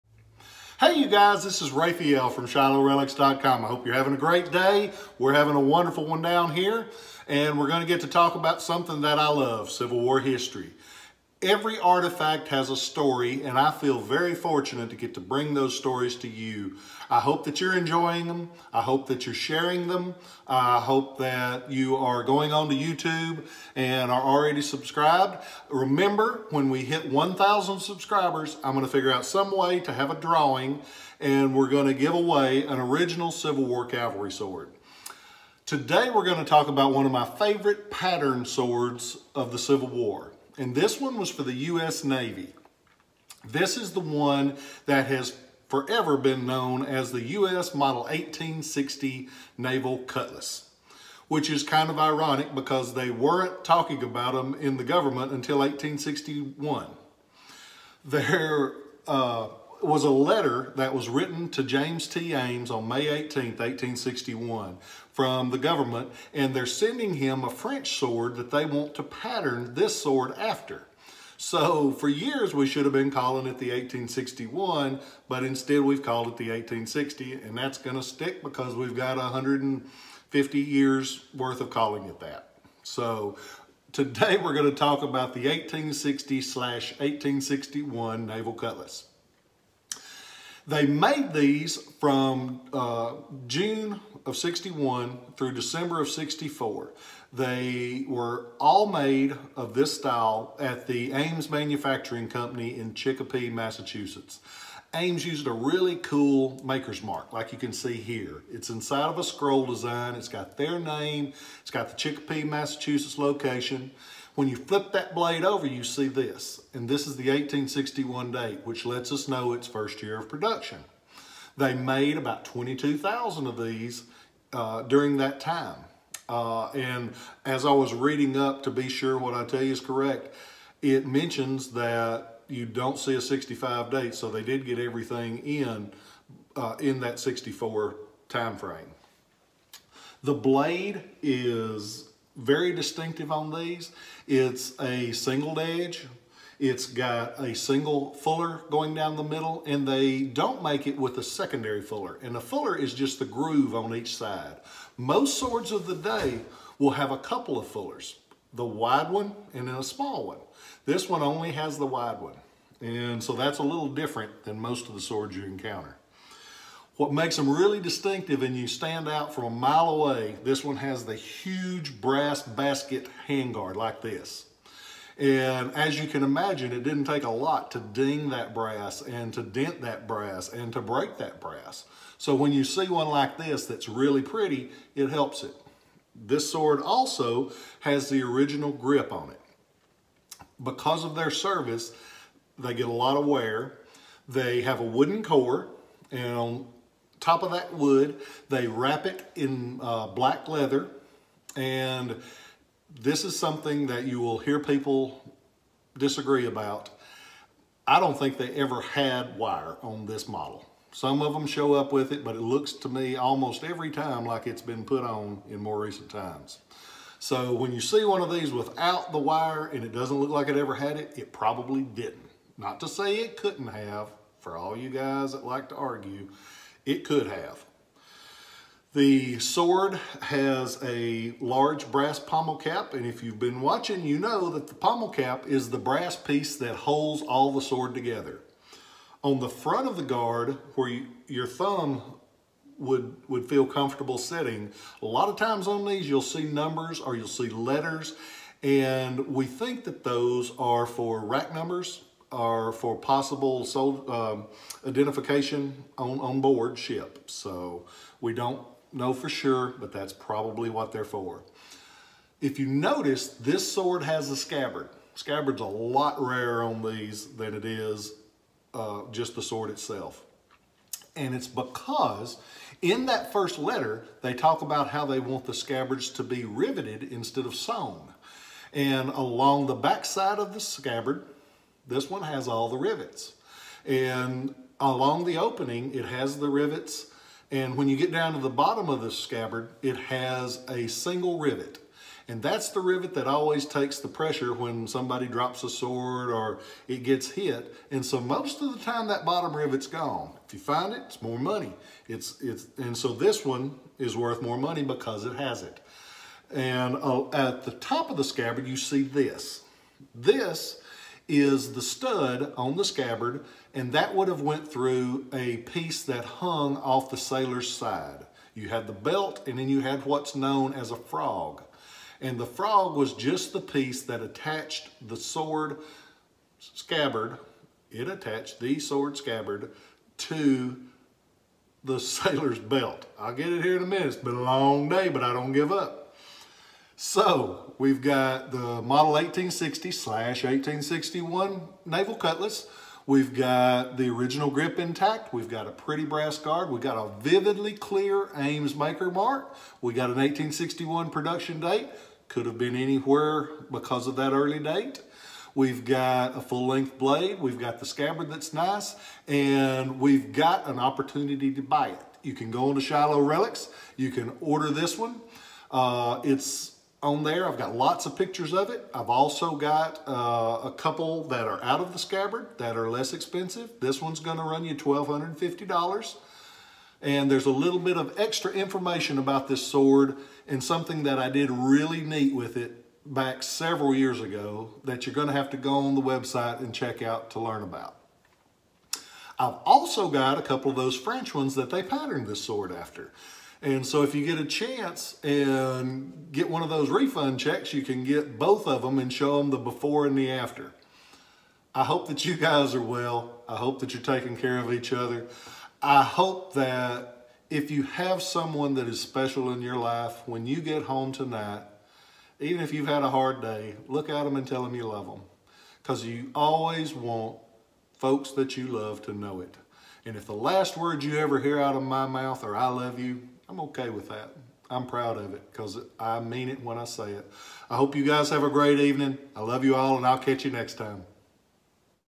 Ames Made Civil War Naval Cutlass with Scabbard
Informational Video by Rafael Eledge about the Civil War Model 1860 Naval Cutlass with Scabbard